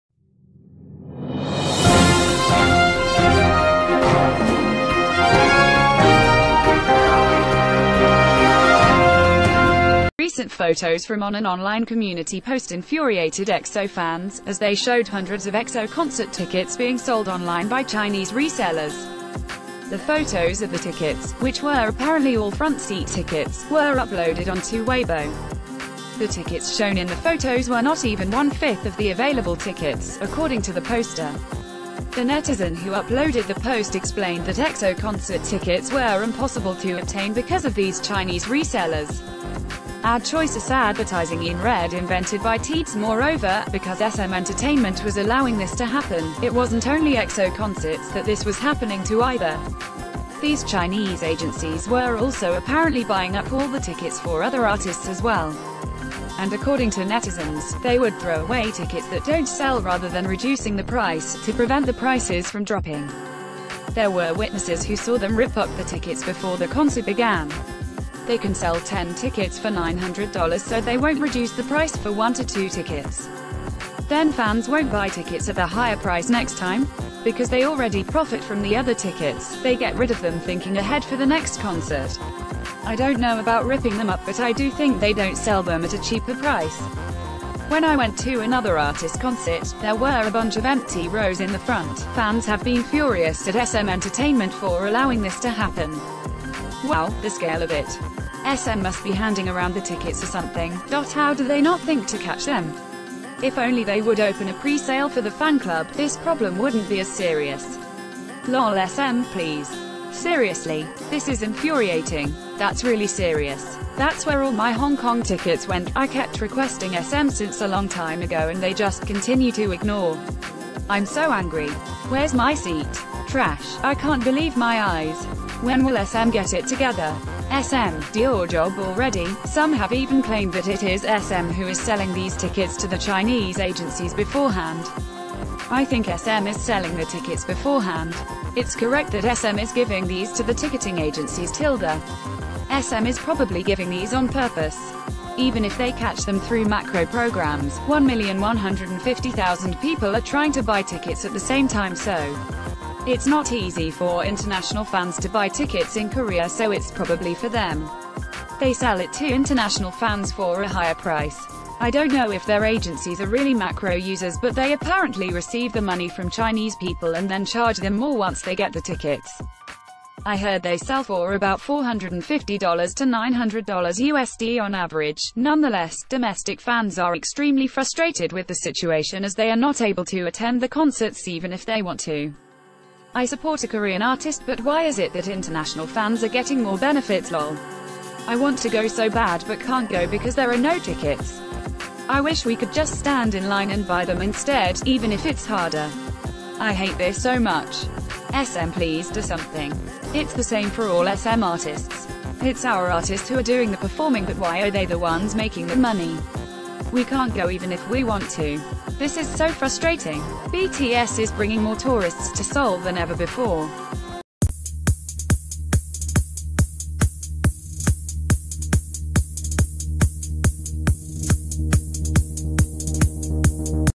EXO-L Are Furious At SM Entertainment For Allowing Chinese Agencies To Buy Up All Of EXO’s Concer...
EXO-L Are Furious At SM Entertainment For Allowing Chinese Agencies To Buy Up All Of EXO’s Concert Tickets
Recent photos from on an online community post infuriated EXO fans, as they showed hundreds of EXO c...